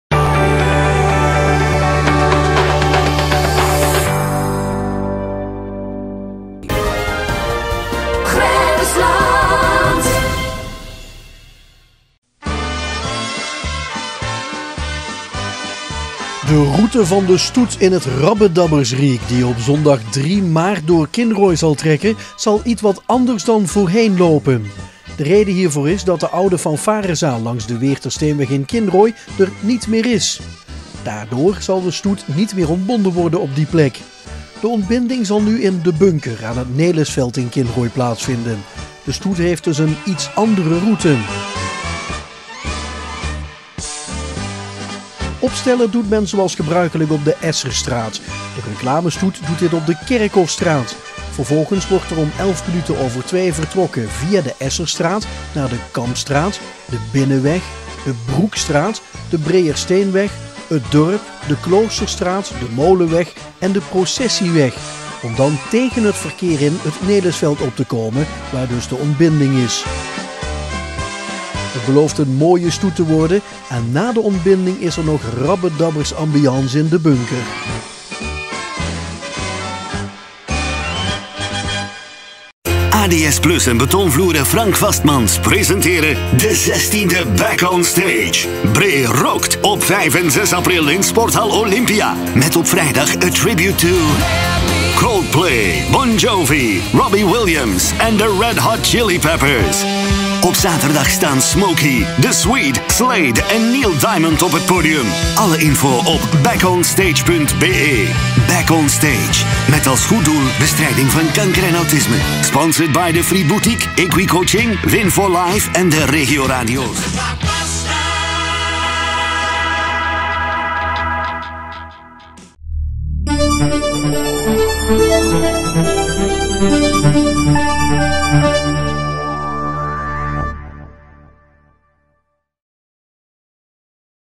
GA Reportage 01032019 Optocht Kinrooi gewijzigd
De route van de stoet die op carnavalszondag, 3 maart, door Kinrooi zal trekken, zal ietwat anders dan voorheen lopen.
De reden hiervoor is dat de oude fanfarezaal langs de Breeërsteenweg in Kinrooi er niet meer is. Daardoor kan de stoet niet meer ontbonden worden op die plek.
De ontbinding zal nu in De Bunker aan het Nelisveld plaatsvinden.
Opstellen doet men zoals gebruikelijk op de Esserstraat. De reclamestoet doet dit op de Kerkhofstraat. Vervolgens wordt er om 14.11 uur vertrokken via de Esserstraat, naar de Kampstraat, de Binnenweg, de Broekstraat, de Breeërsteenweg, het Dorp Kloosterstraat, Molenweg en Processieweg om dan tegen het verkeer in het Nelisveld op te komen waar de ontbinding is.
Het belooft een mooie stoet te worden en na de ontbinding is er nog ambiance in De Bunker.